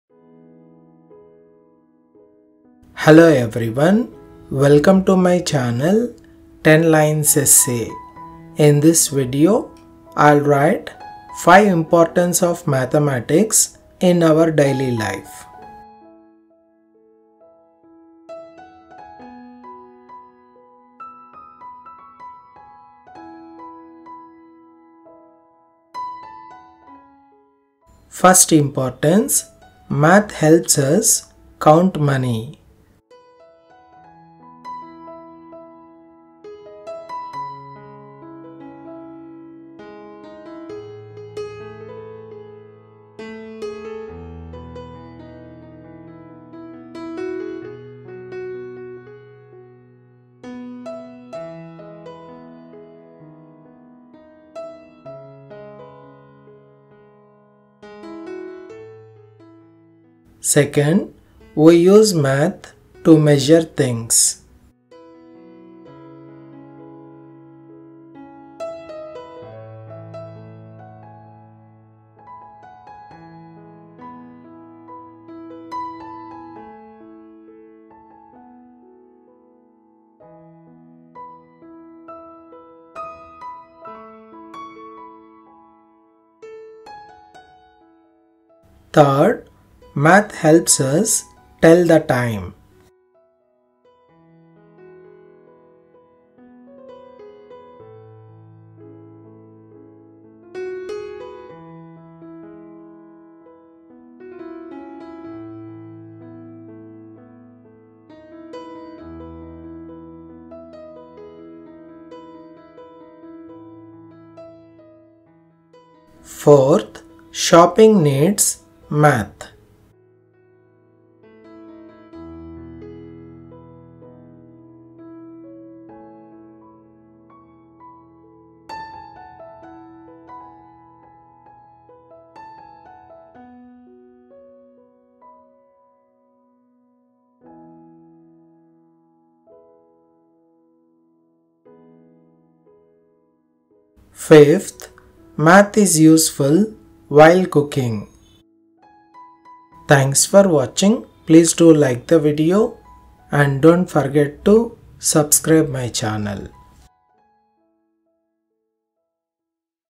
Importance of Mathematics in Our Daily Life | Five Lines on the Importance of Maths in Daily Life
Would you like to know the importance of Mathematics in our daily lives?  If so, you have come to the right place!
This video discusses the importance of mathematics in our daily lives.  This is an updated video with five importance of mathematics in our daily life. It will help you gain knowledge and awareness about the topic.

Importance of Mathematics in our daily life